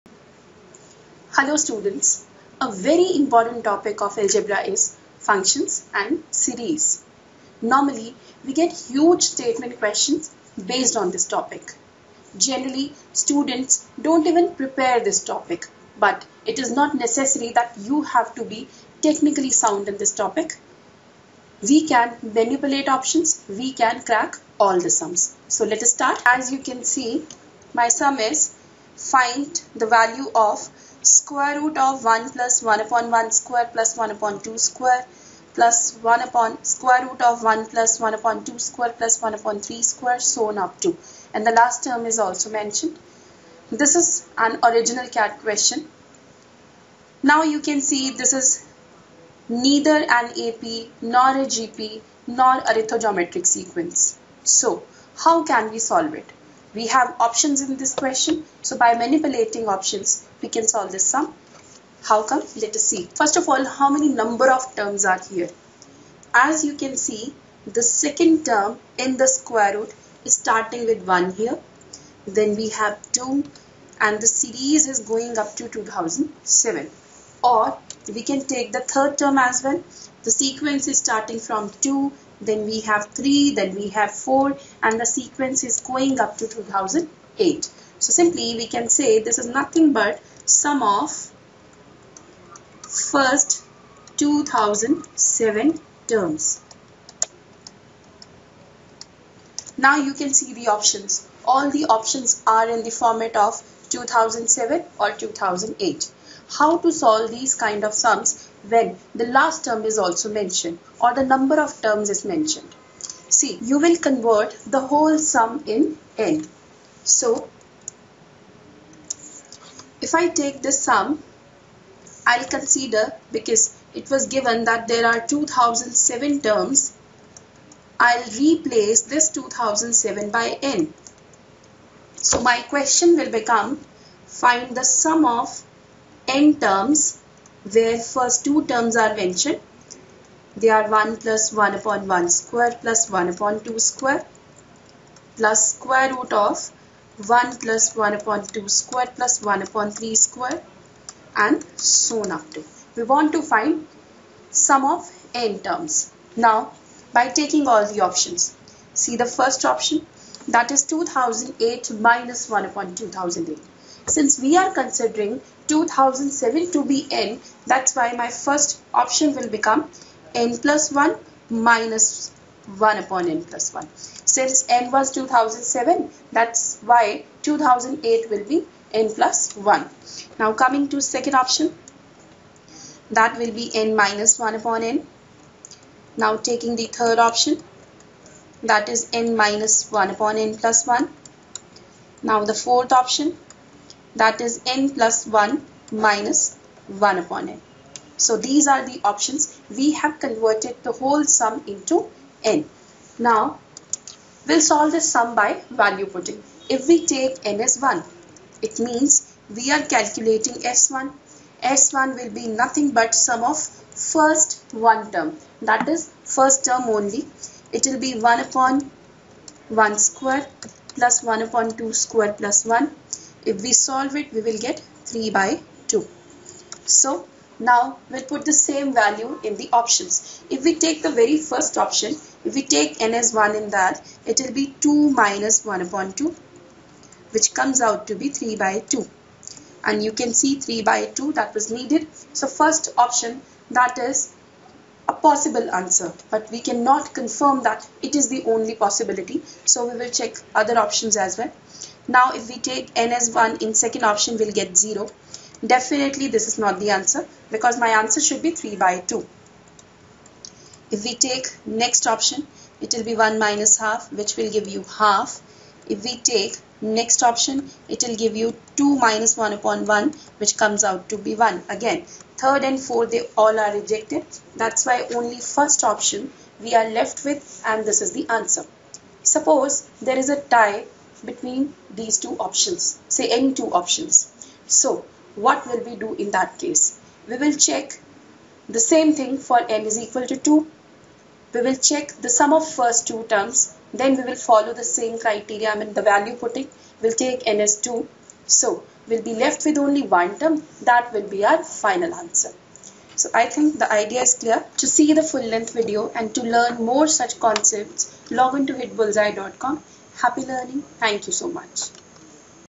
Functions - Under One Minute
Learn the easy  trick of manipulating the options and cracking all questions in  this topic FRACTIONS.